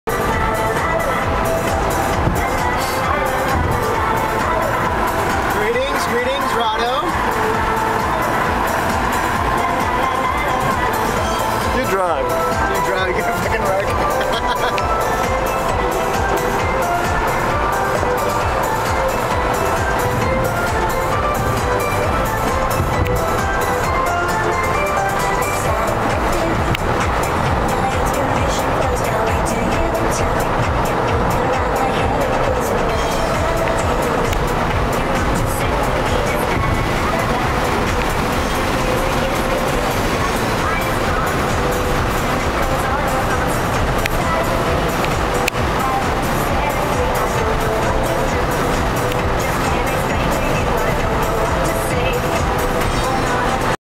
SoCal drive..
Driving down the highway towards San Diego..